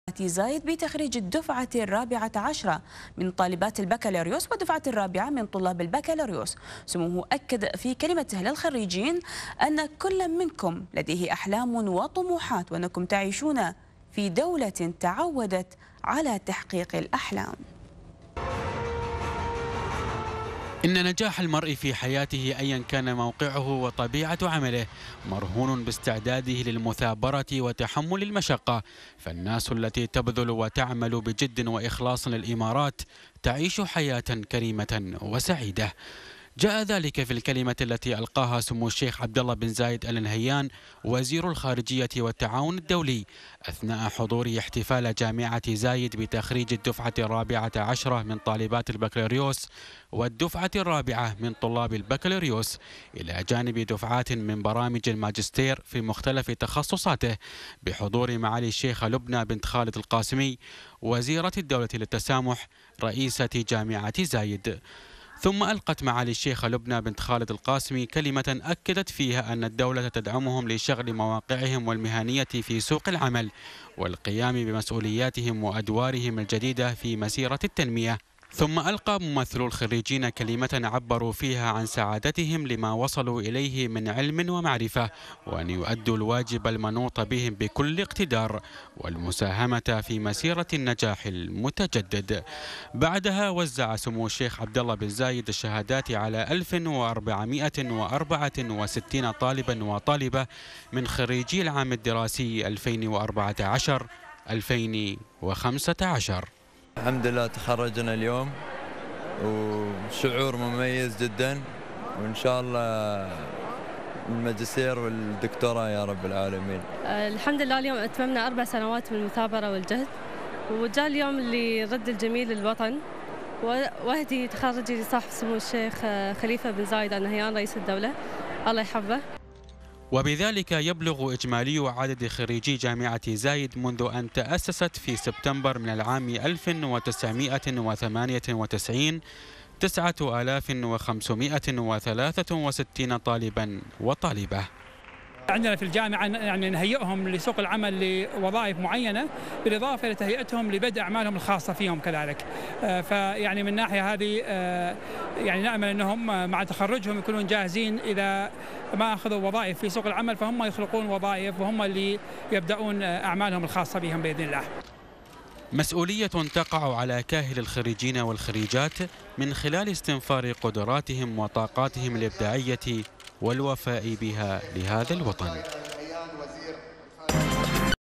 سمو الشيخ عبدالله بن زايد يشهد تخريج جديد بجامعة زايد بحضور الشيخة لبنى القاسمي وسعادة مبارك الشامسي
مع تحياتي .عادل عرفة